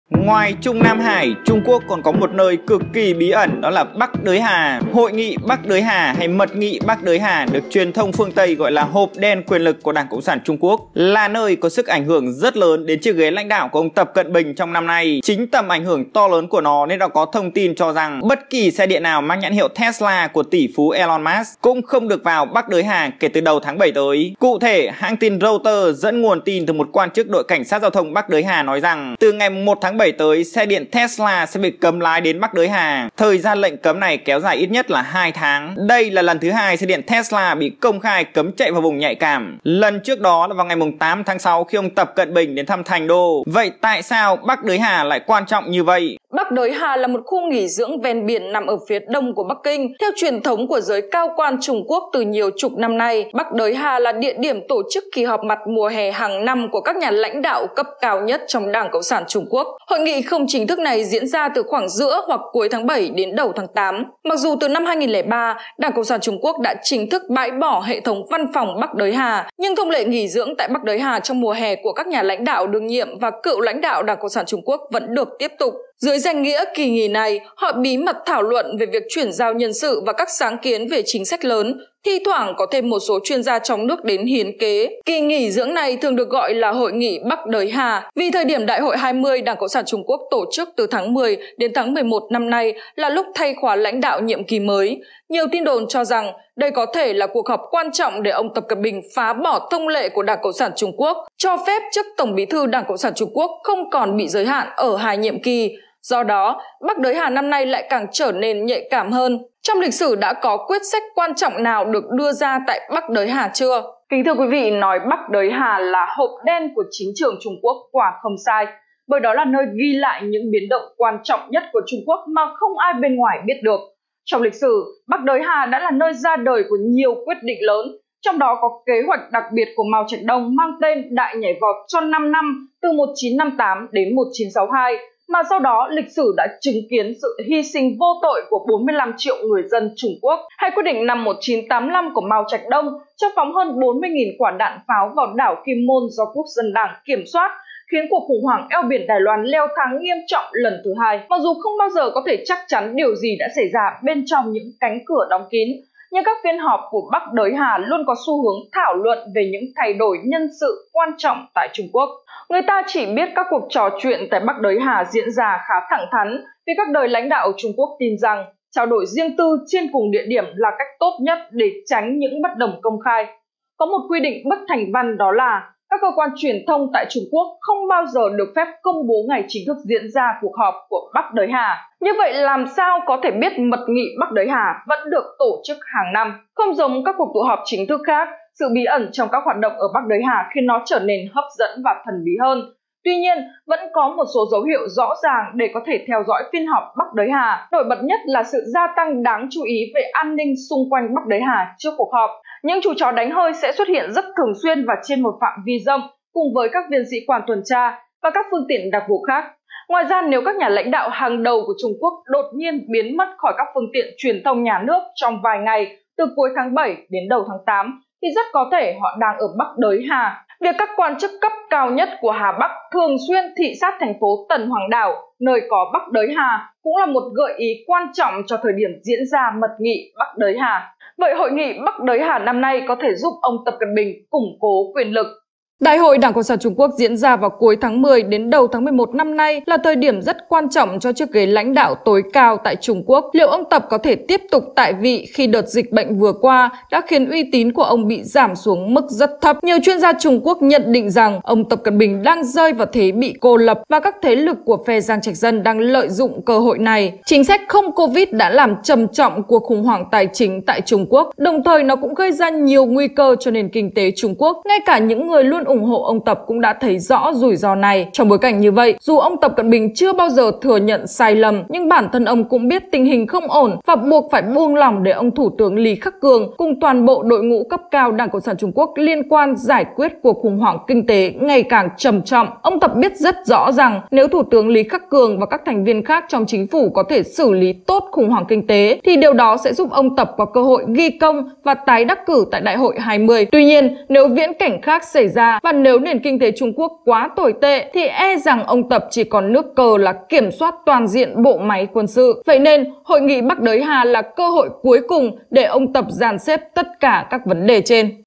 Hội nghị BÍ ẨN Bắc Đới Hà, liệu sẽ ĐỊNH ĐOẠT chiếc ghế của ông Tập? - Tin Cập Nhật
Ngoài Trung Nam Hải, TQ còn có một nơi cực kỳ bí ẩn đó là Bắc Đới Hà
Hội nghị Bắc Đới Hà hay Mật Nghị Bắc Đới Hà, được truyền thông phương tây gọi là ‘HỘP ĐEN QUYỀN LỰC’ của ĐCSTQ, là nơi có sức ảnh hưởng RẤT LỚN đến chiếc ghế lãnh đạo của ông Tập Cận Bình trong năm nay

Chính tầm ảnh hưởng to lớn của nó, nên đã có thông tin cho rằng, bất kỳ xe điện nào mang nhãn hiệu Tesla (của tỷ phú Elon Musk) cũng không được vào Bắc Đới Hà kể từ đầu tháng 7 tới